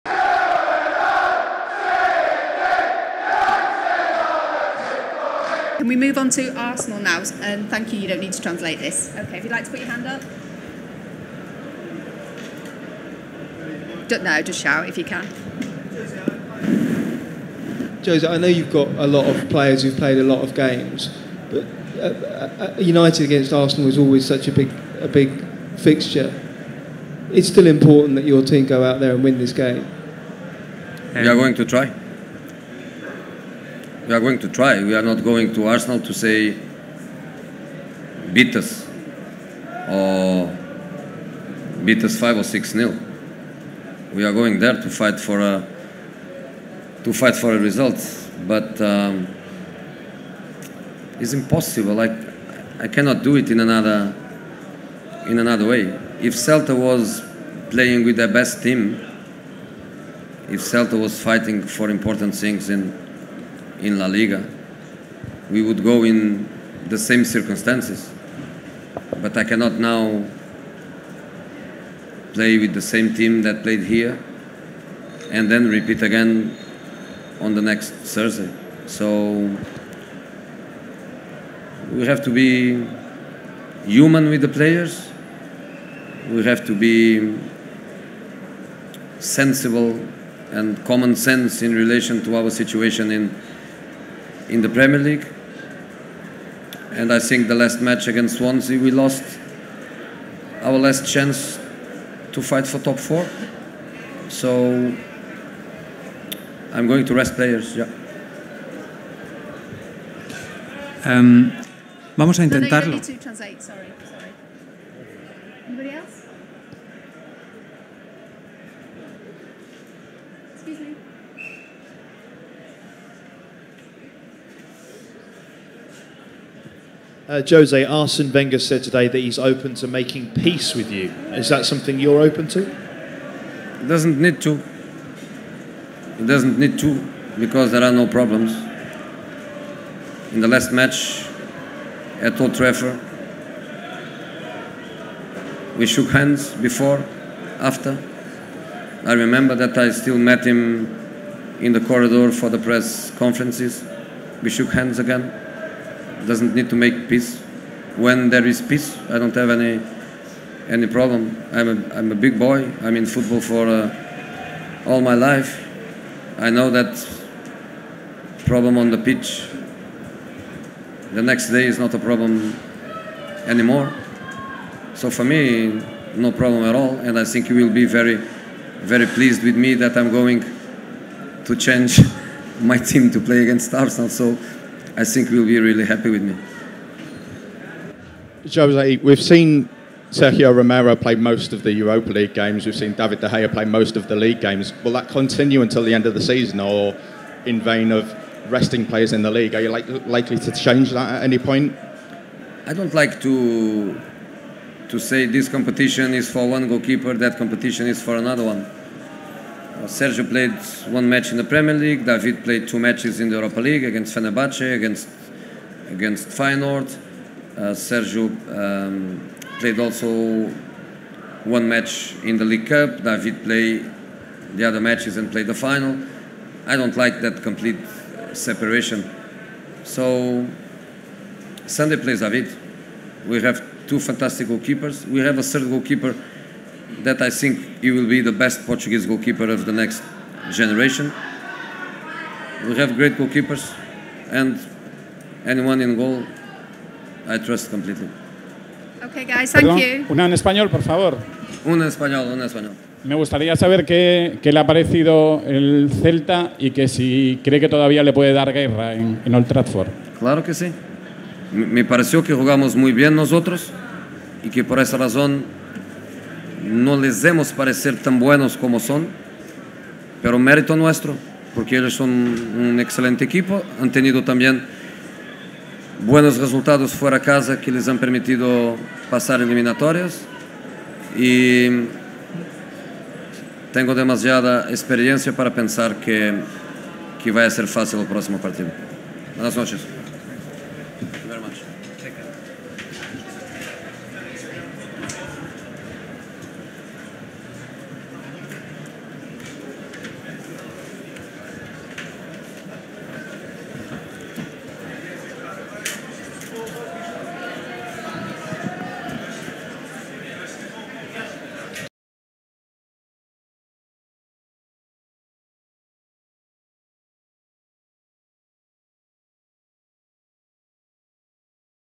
Jose Mourinho Full Pre-Match Press Conference - Arsenal v Manchester United
Thanks